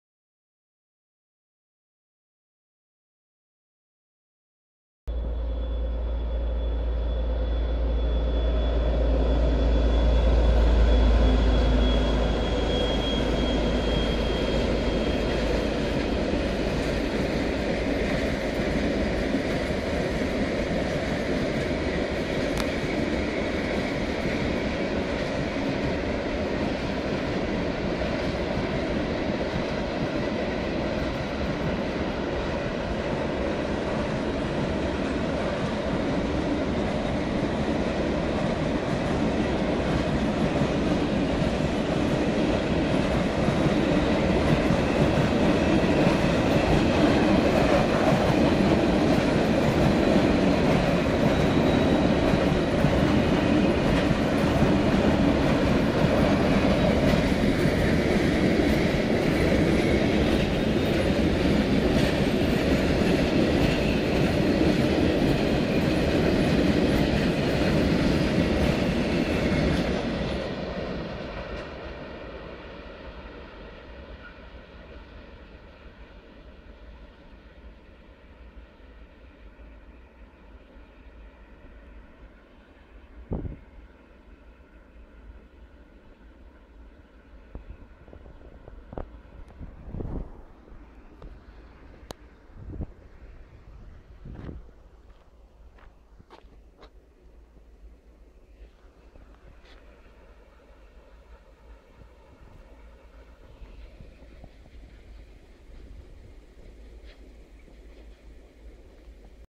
ST44-313 na przejeździe z węglarkami
Gagar z węglarkami przejeżdża przez przejazd kolejowy we wsi Franopol w Sarnakach.